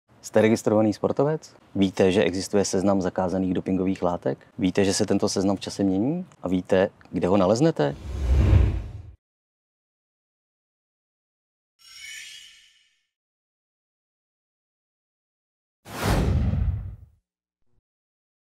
Sportovci v právu - Seznam zakázaných látek
Vzdělávací kurz "Sportovci v právu"
- kurz sportovního práva

Přednášející: Mgr. Martin Čížek, LL.M.
- právník UK FTVS, advokátní praxe, specializace sportovní právo

Tématické okruhy:
- hráčská smlouva a z ní plynoucí práva a povinnosti
- zákoník práce a sport
- rasismus ve sportu
- match fixing
- doping